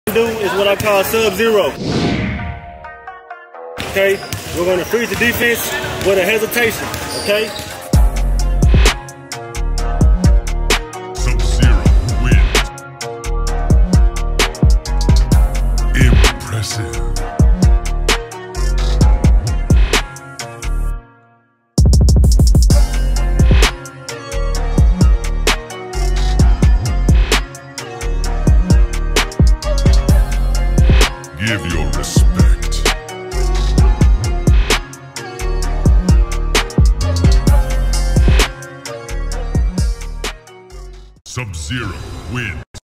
Sub Zero move aka hesitation taught by Basketball Guru Beal
The hesitation move can be used to freeze your defender then you have the advantage..

For more content visit Gamewinnerllc.com